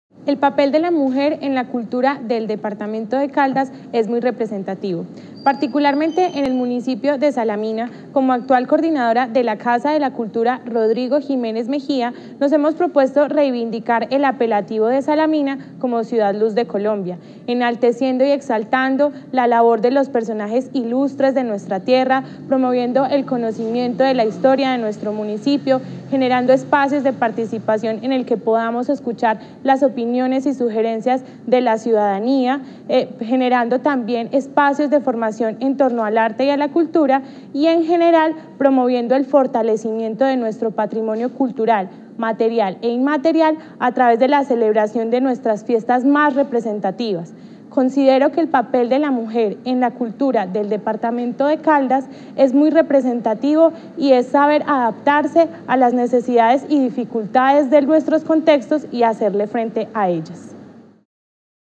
La mujer en la cultura¡Feliz día mujeres!
La mujer en la cultura
¡Feliz día mujeres!
Juventud, experiencia y gestión en la cultura caldense!
Casa de la Cultura "Rodrigo Jiménez Mejía", Salamina Casa De La Cultura Salamina Caldas